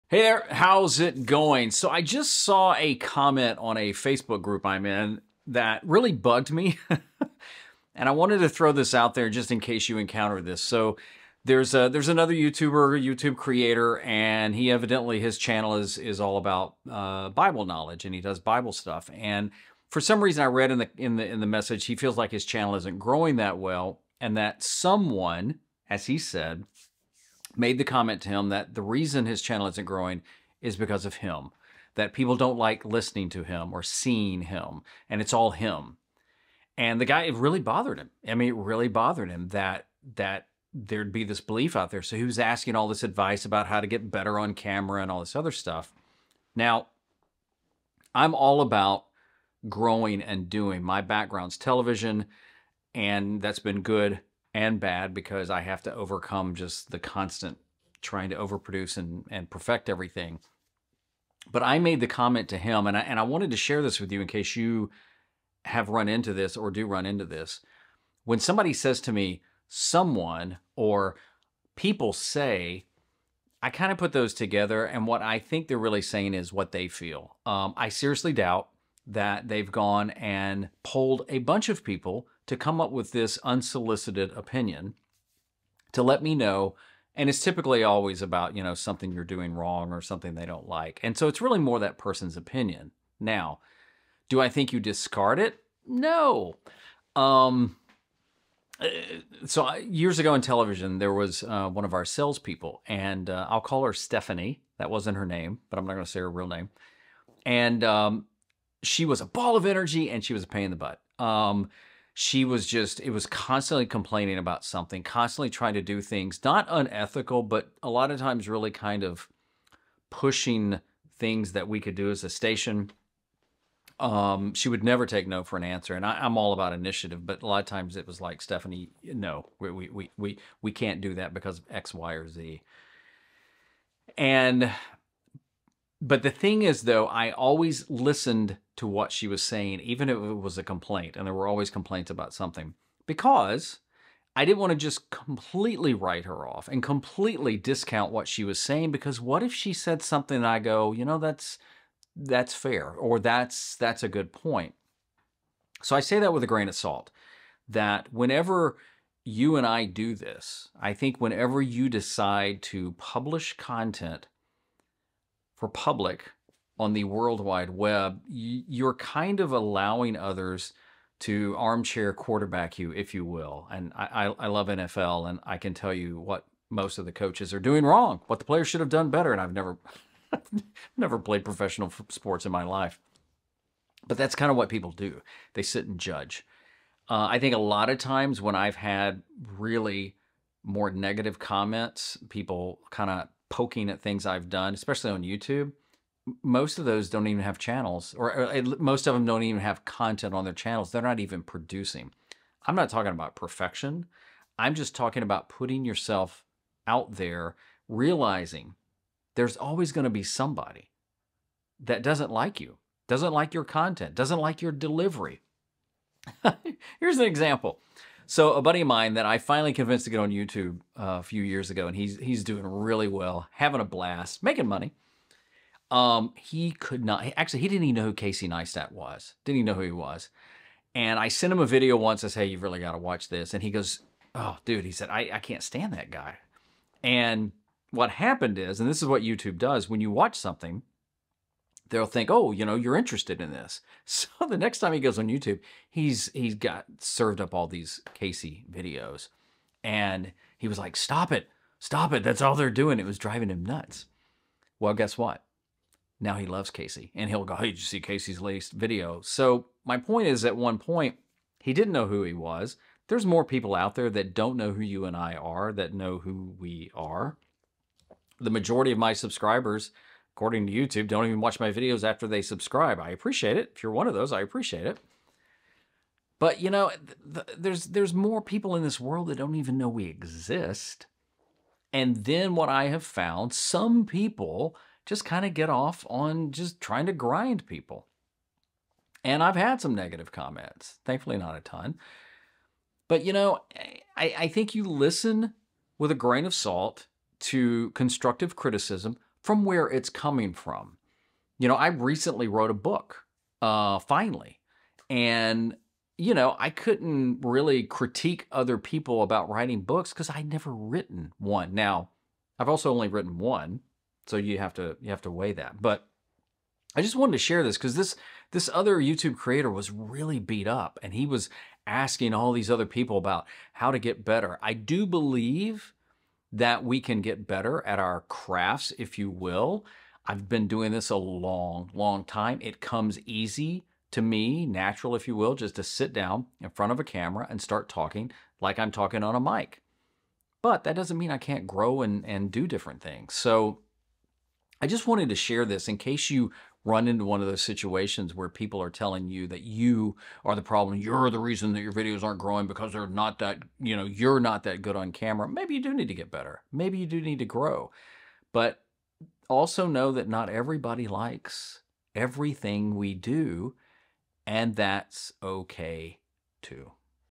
when people don't like you
What do you do when someone doesn't like you as a creator?